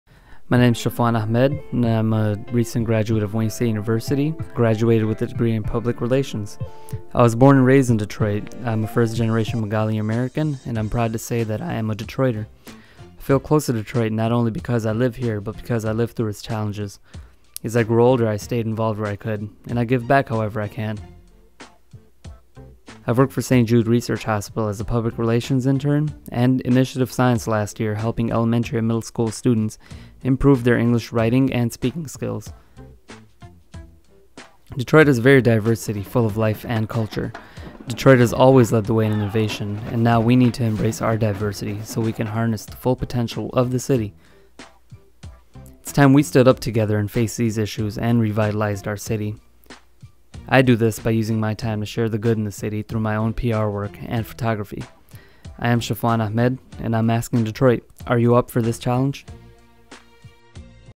Challenge Detroit - Shaffwan
This is my video submission for Challenge Detroit.

PLEASE Vote for me! You can only vote through "Top Tab" on Facebook

Thank you to Shahjahan for everything you did, I cannot thank you enough.